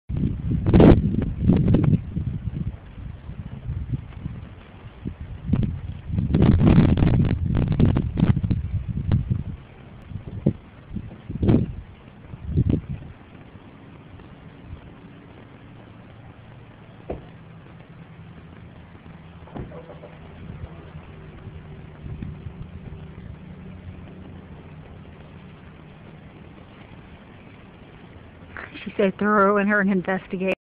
Tractors are Scary
Oh TOTALLY
(During Nike's first walk around the barn)